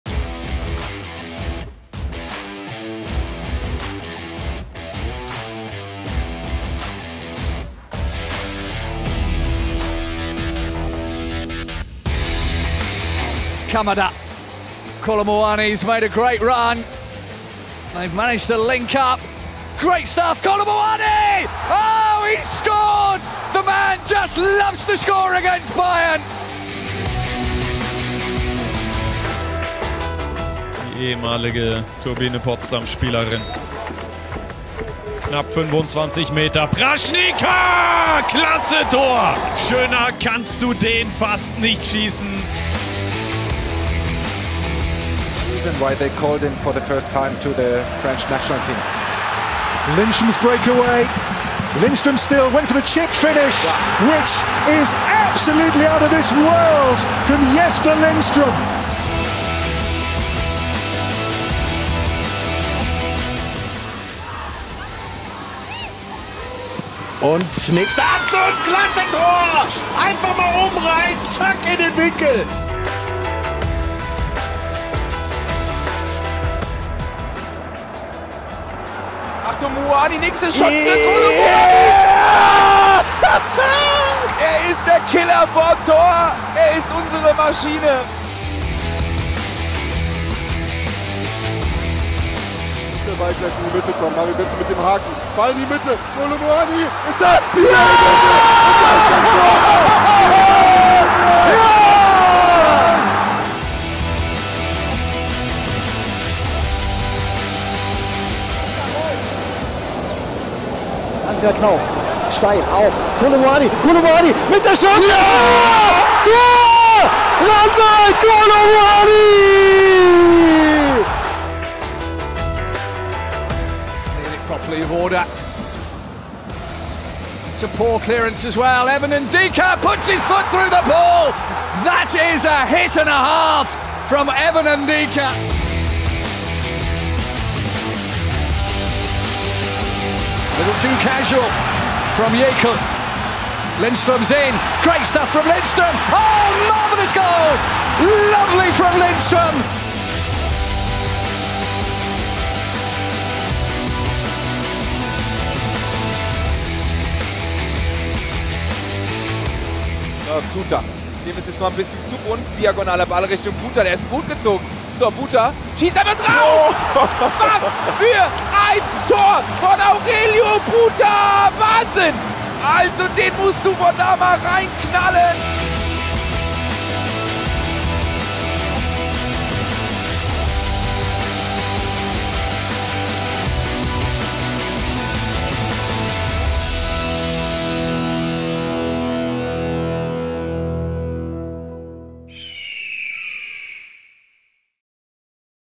Kolo Muani, Buta & Co. I Die Top-10-Tore der Saison 2022/2023
In der abgelaufenen Saison durften wir uns wieder an einigen wichtigen und schönen Toren erfreuen. Wir haben unsere Top10 für euch zusammengefasst!